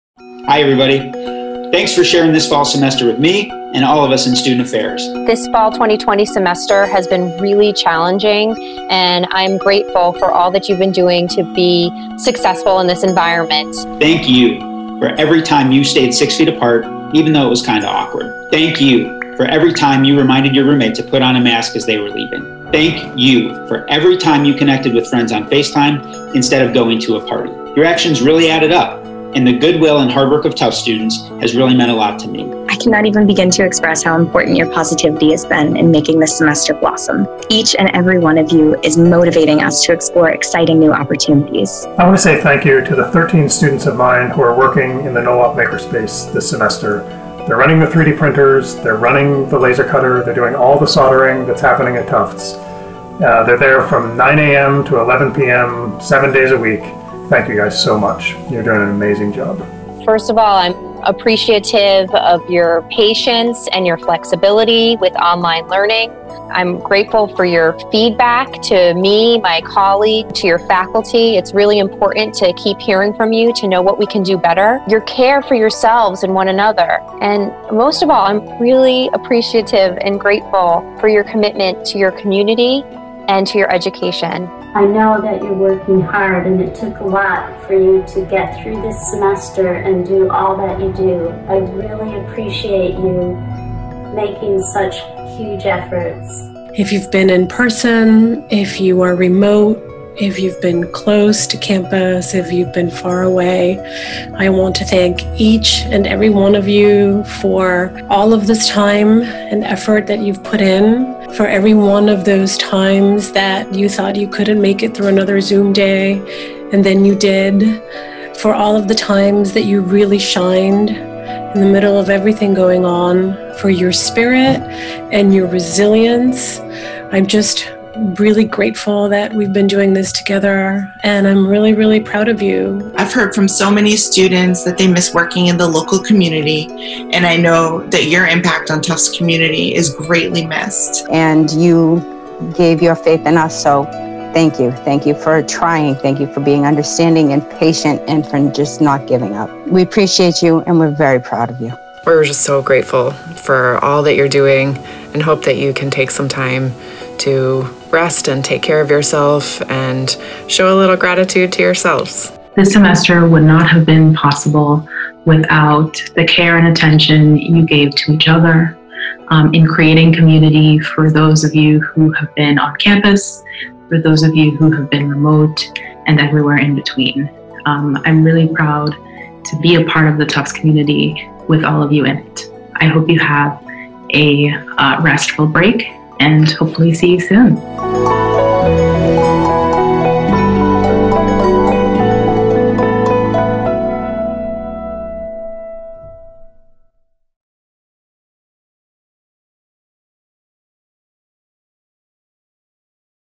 Tufts students... thank you!
As the Fall 2020 semester concludes, a message of gratitude for the students of Tufts.